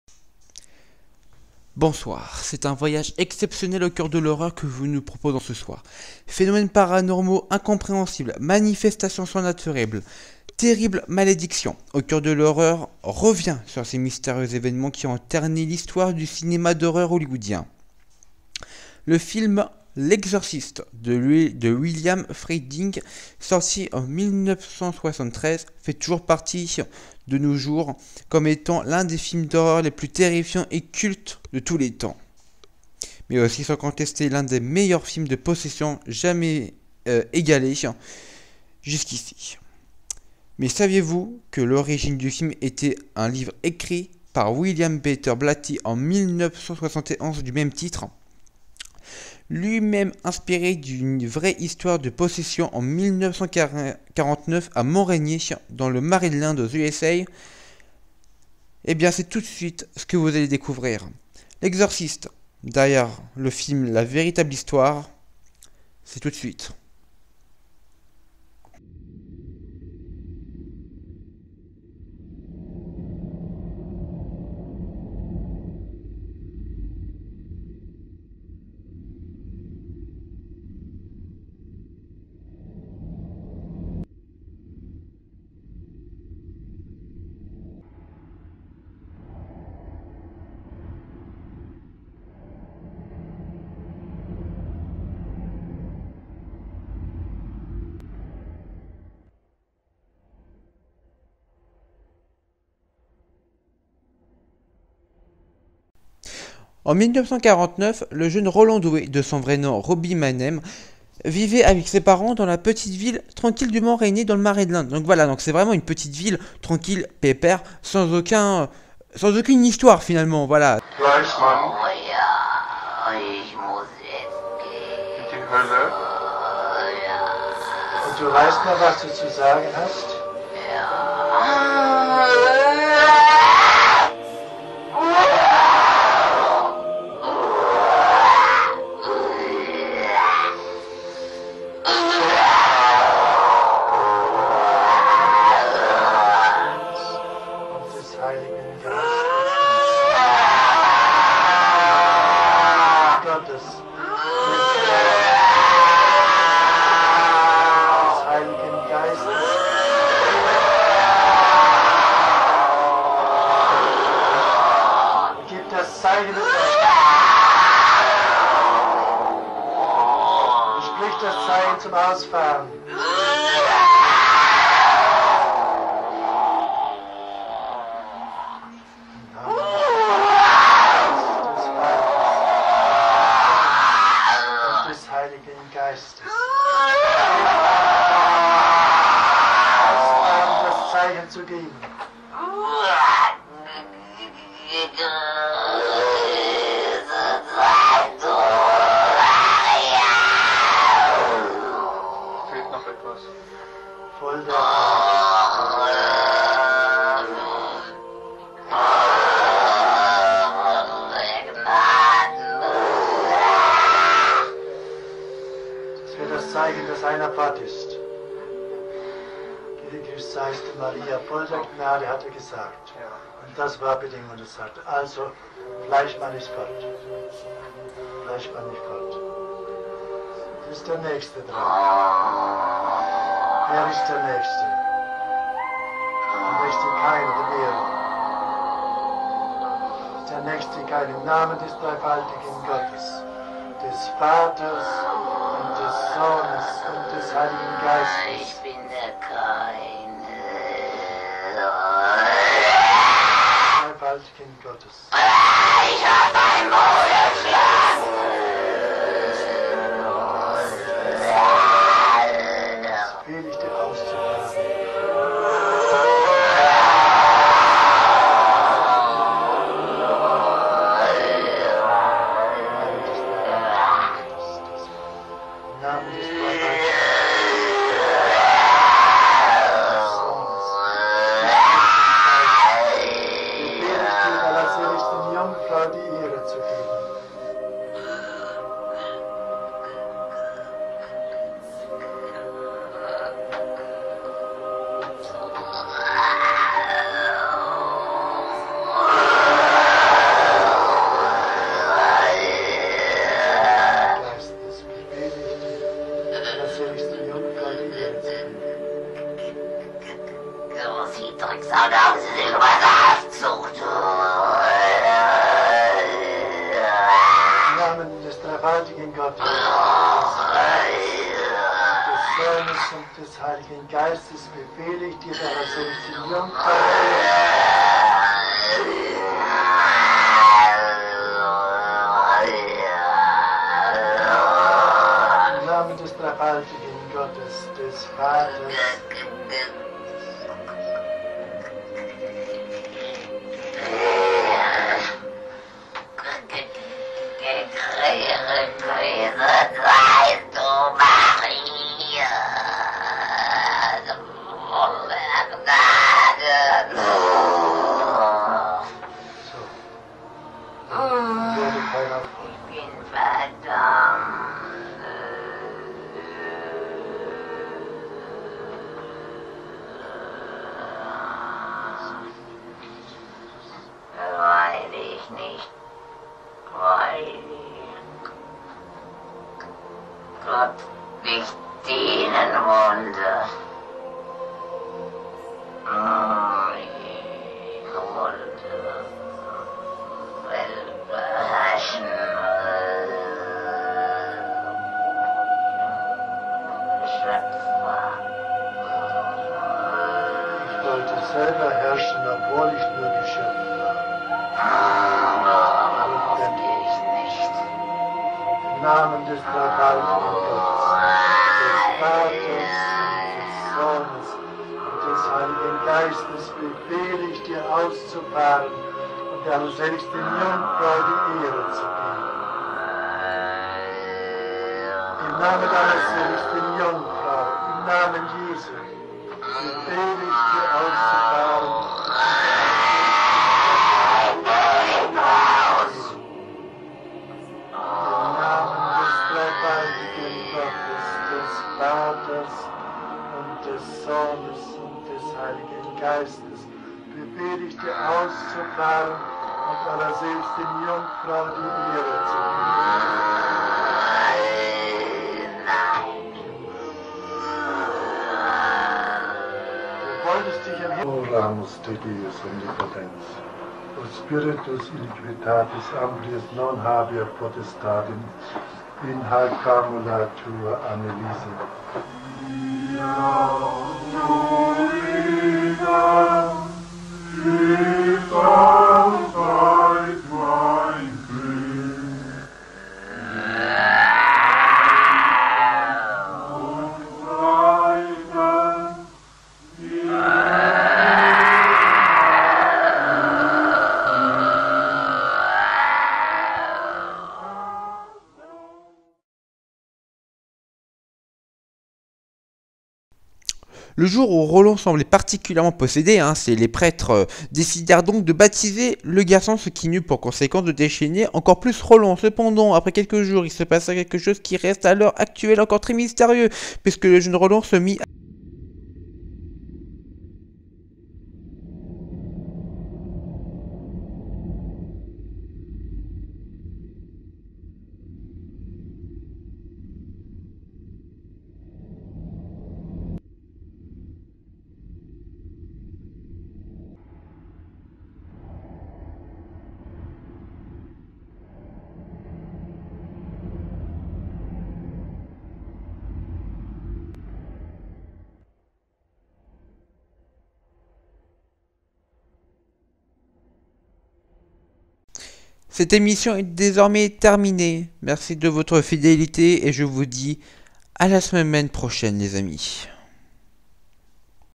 AU CŒUR DE L'HORREUR - Derrière le film, la VÉRITABLE histoire...
Bonsoir,
Phénomènes paranormaux incompréhensible, manifestations surnaturelles, terribles malédictions... "AU CŒUR DE L'HORREUR" revient sur ces mystérieux événements qui ont ternie l'histoire du cinéma Hollywoodien.

Rendez-vous le jeudi 17 octobre à 21:00 pour l’épisode 3 !

Une production CritizProd'.